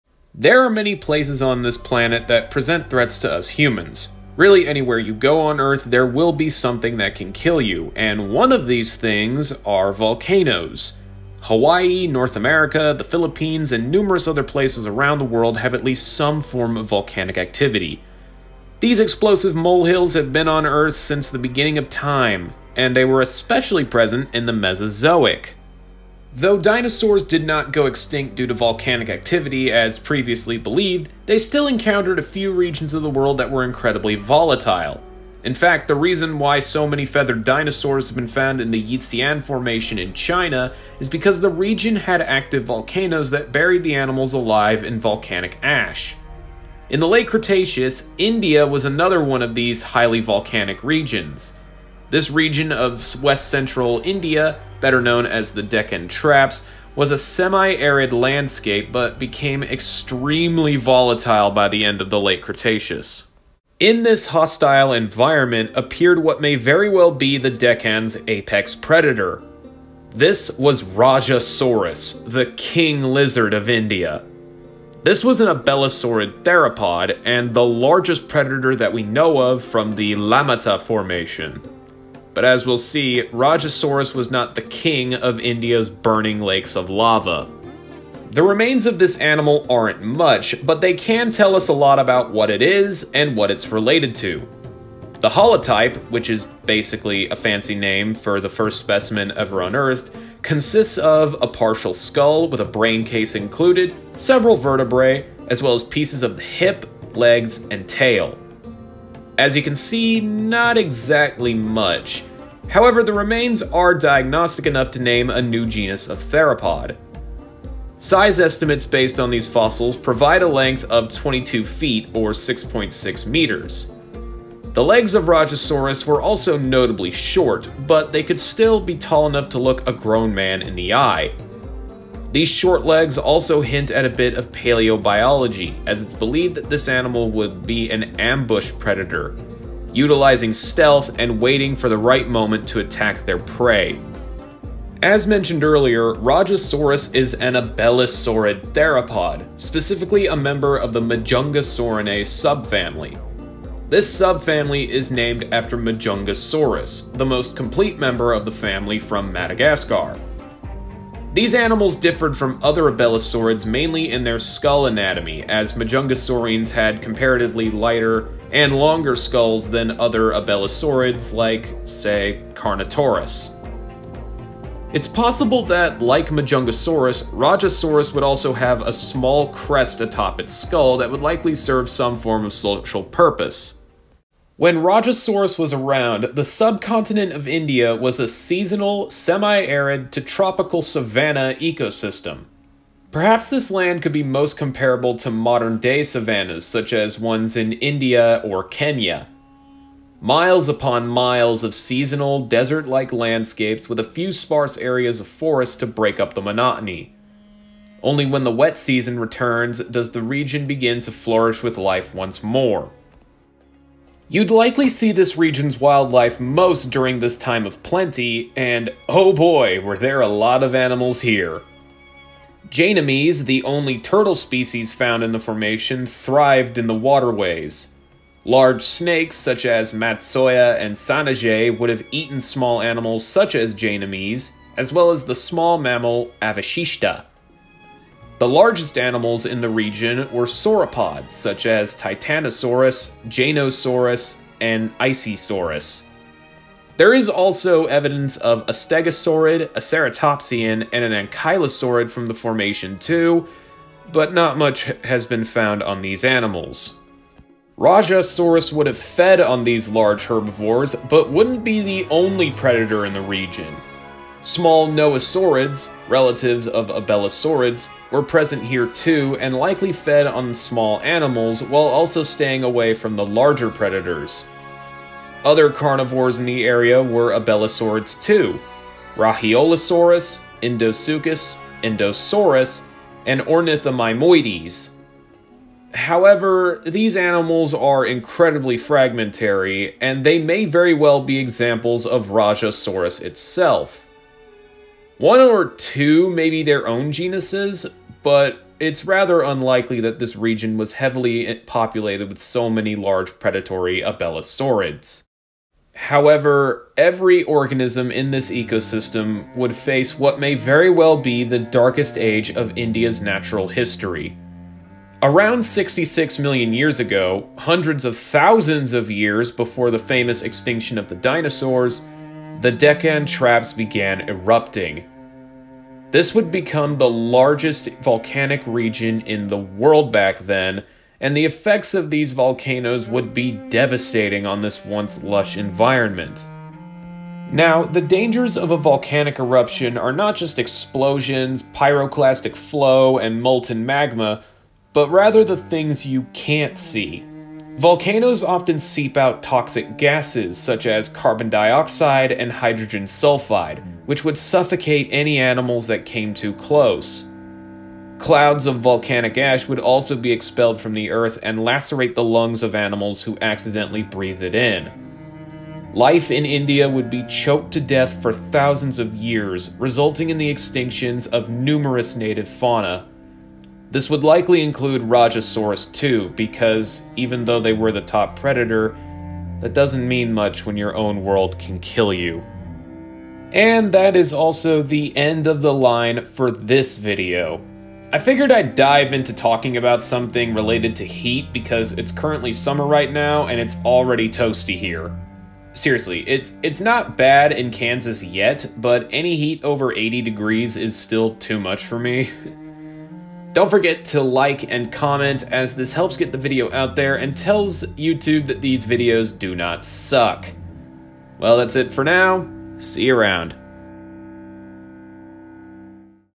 Rajasaurus: The King of India’s Volcanic Lands
Rajasaurus is such a magnificent animal and their extinction is almost a tragic tale written like a Greek play. I’ve been wanting to talk about these animals for a while and I was so happy that I was able to!

Info in this video came from
Wikipedia
HenryThePaleoGuy - Rajasaurus: King Of India